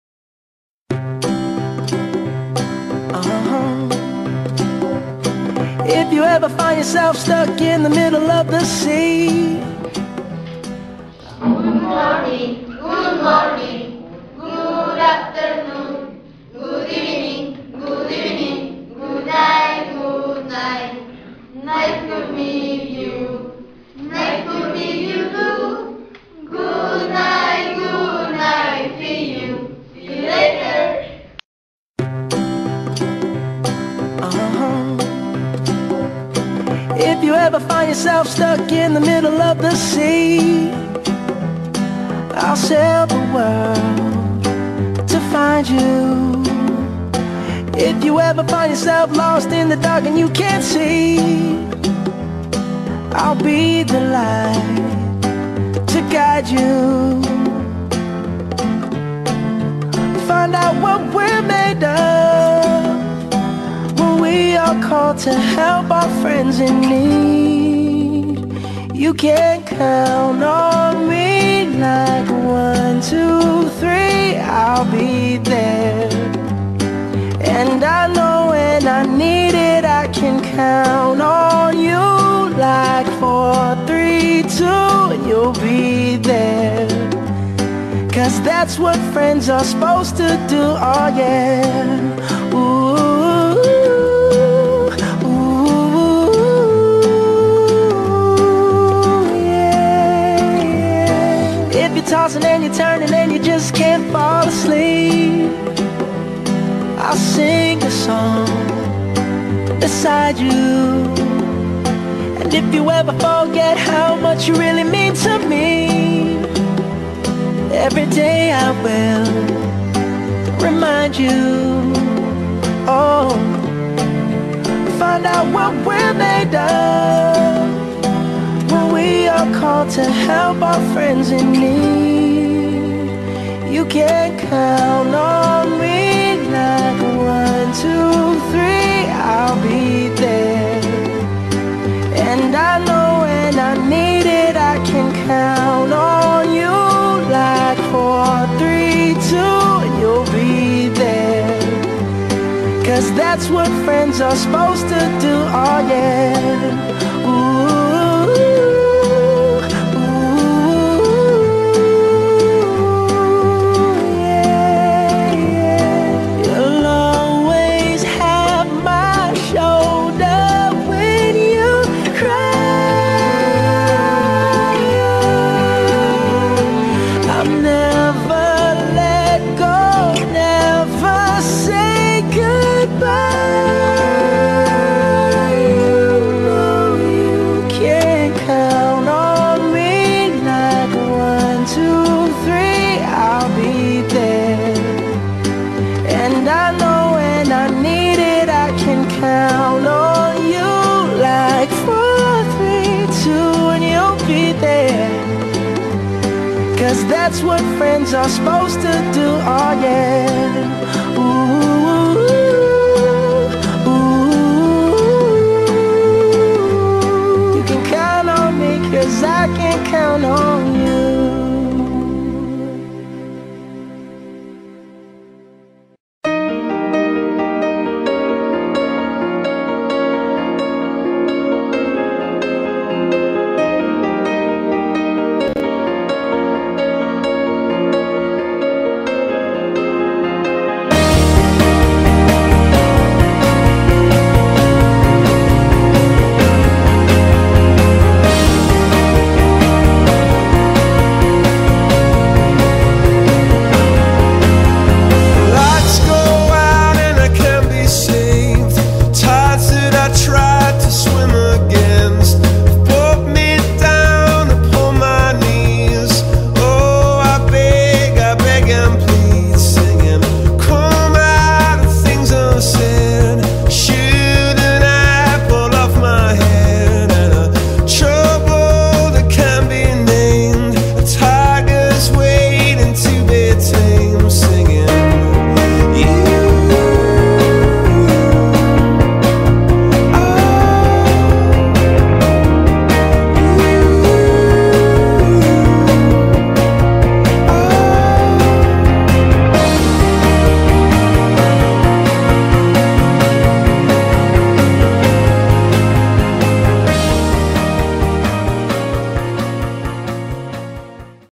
Presentación clase de Inglés 2013 - Liceo Bicentenario Valdivia
Esta es una presentación hecha para la muestra de talleres a finales del año pasado en nuestro Liceo Bicentenario Ciudad de los Rios (Valdivia, Chile). Más que una apretada presentación de las actividades más importantes del año, son emotivos y gratos recuerdos vividos juntos a nuestros alumnos.